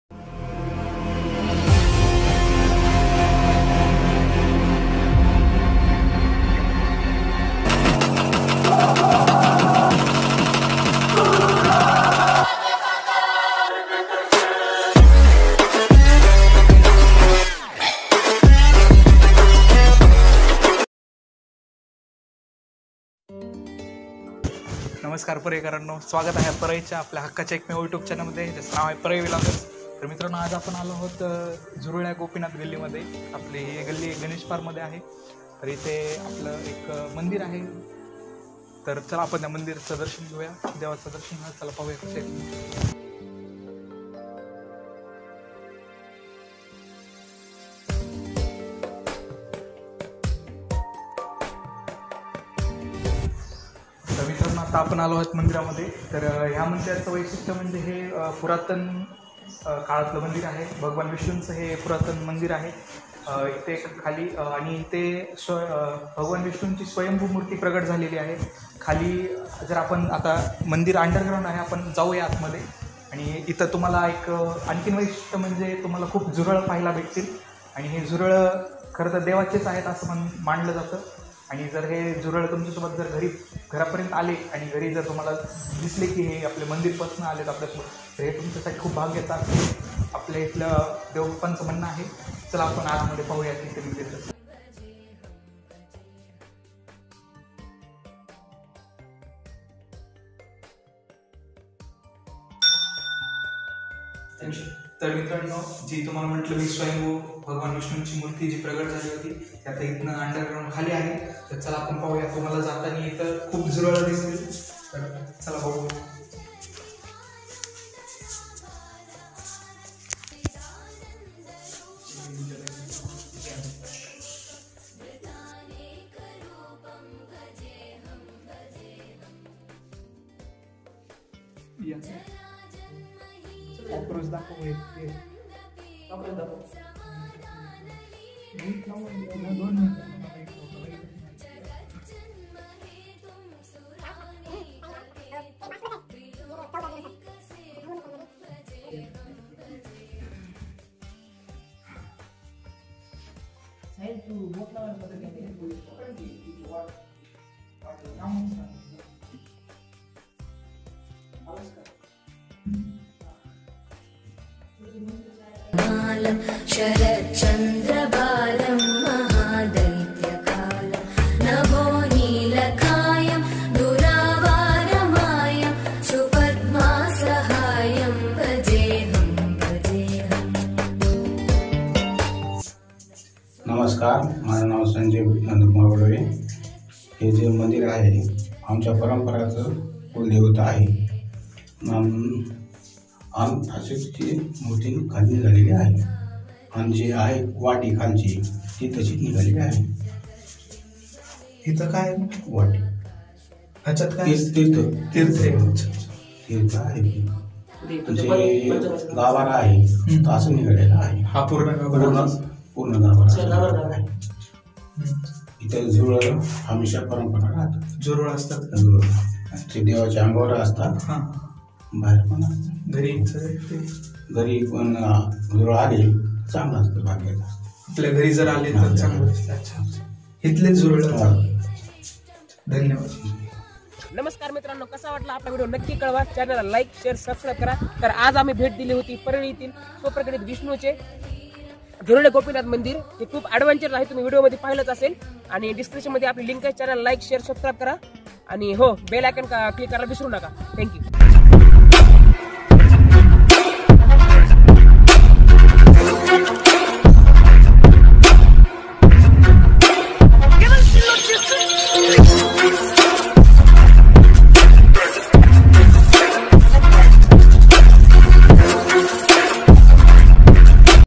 परळीतील प्राचिनतेचे प्रतीक ||Parli V Loggers
परळी वैजनाथ शहरात गणेशपार भागामध्ये झुरल्या गोपीनाथ गाली मध्ये वसलेले भगवान विष्णू चे एक प्राचीन मंदिर आहे. हिते भगवान विष्णू ची स्वयंभू मूर्ती प्रगटली आहे. हितील वातावरण एकदम शीतल व शांततेचा आहे. तुम्ही सुद्धा एकदा हिते अवश्य भेट द्या आणि भगवान श्री हरी श्री विष्णूचे दर्शन घ्यावे.
Cast - Tejas Arun Rode
shot & Edit - Sahil kamble (the sk editor)